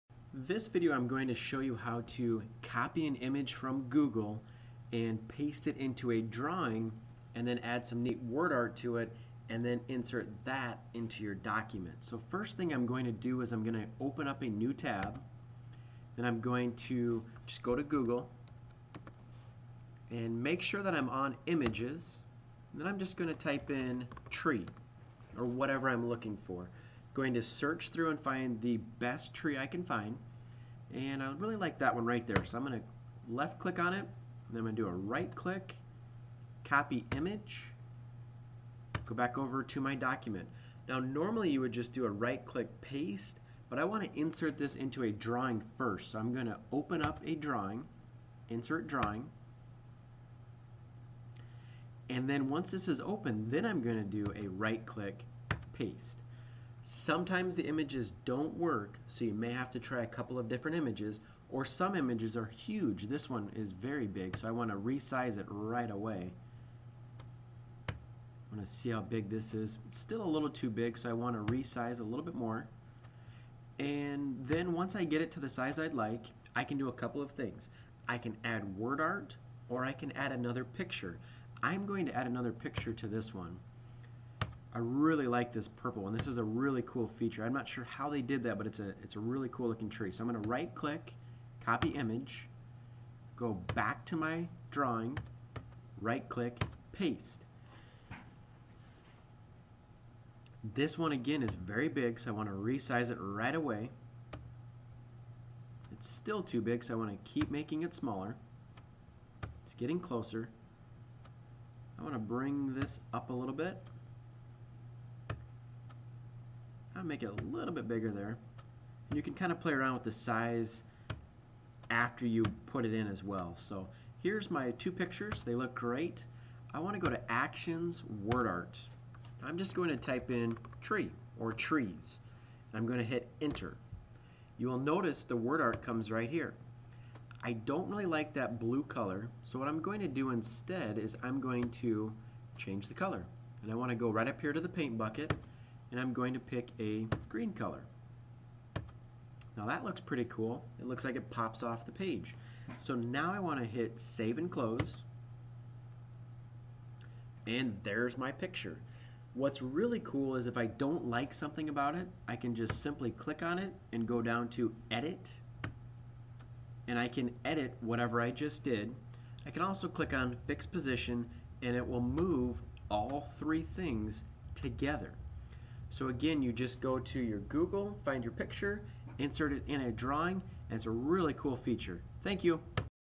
Google Drive-Inserting a drawing
This video will show you how to copy an image from Google and paste it into a drawing.